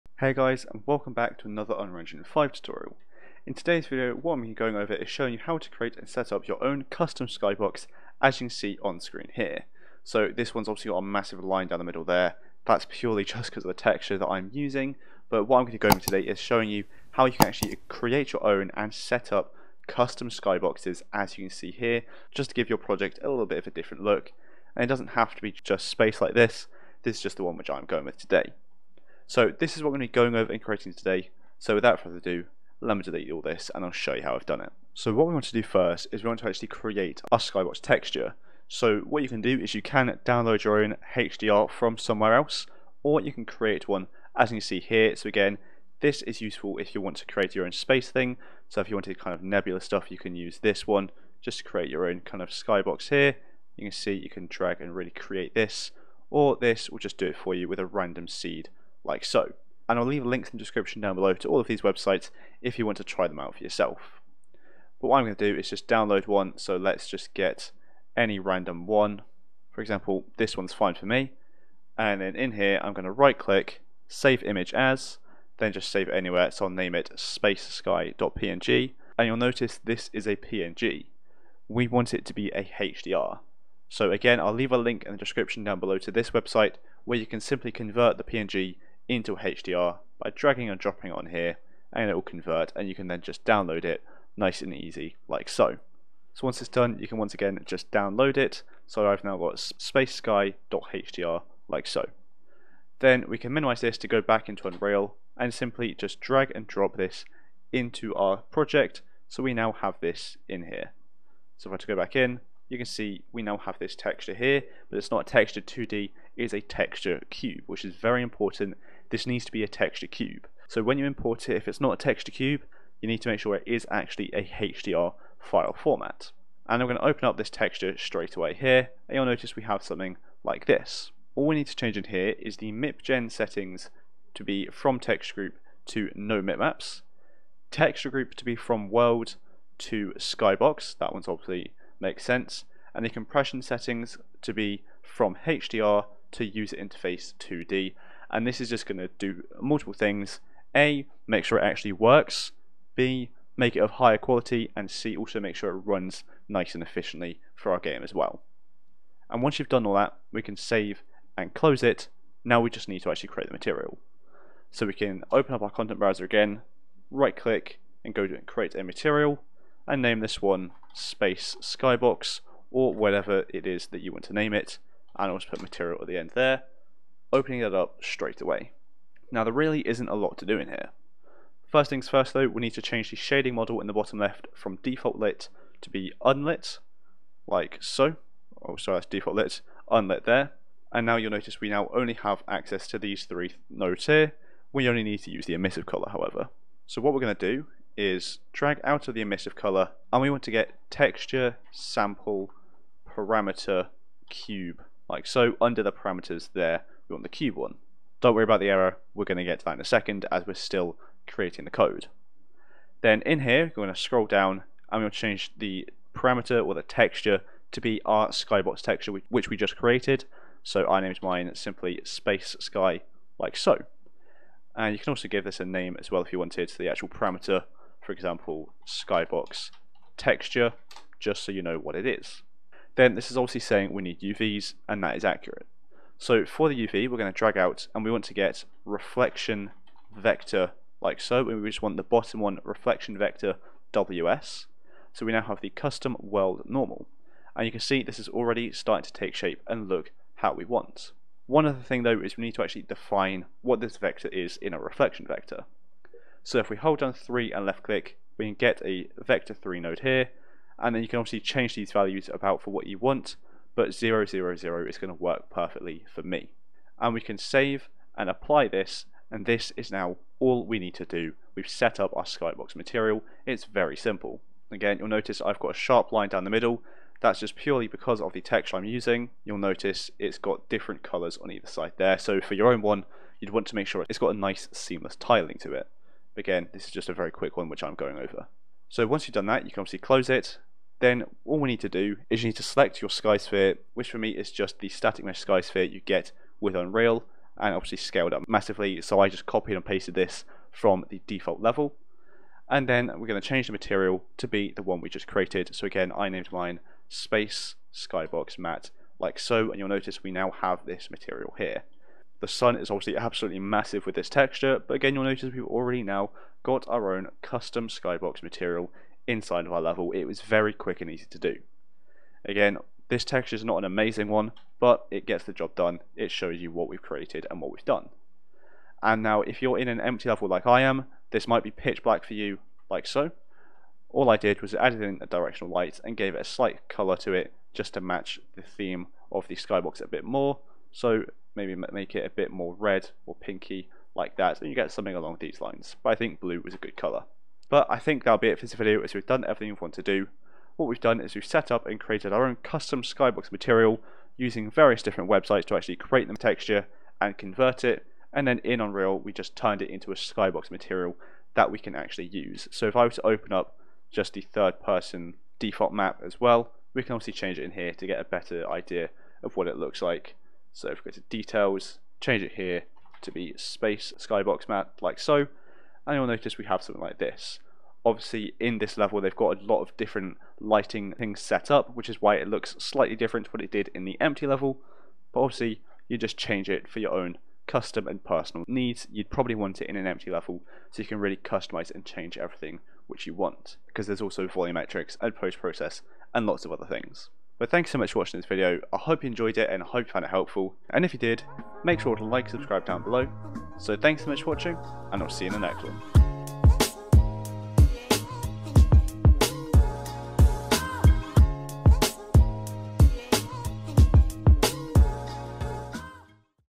How To Create And Use A Custom Sky Sphere In Unreal Engine 5 (Tutorial)
Hey guys, in today's video I'm going to be showing you how to create and use a custom skybox or sky sphere material for your projects inside of Unreal Engine 5. I'm going to be creating a space scene, but you can do whatever you like.

00:00 - Intro
00:08 - Overview
00:34 - Creating The Sky Texture
02:42 - Creating The Skybox Material
04:50 - Using The Skybox In Our Level
05:47 - Final Overview
06:37 - Outro